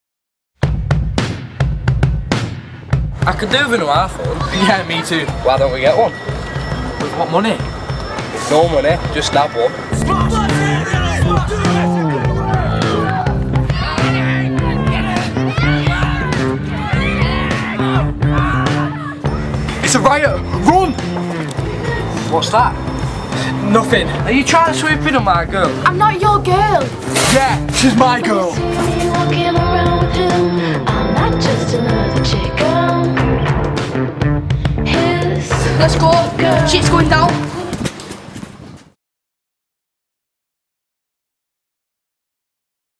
Take/Take [Trailer]
I did a project with BYT back in Easter which focused on the mindset of teens and what they would do if they were in the midst of a riot. This project was inspired by the London Riots made by Burnley Youth Theatre in association with First Light and Seal Films.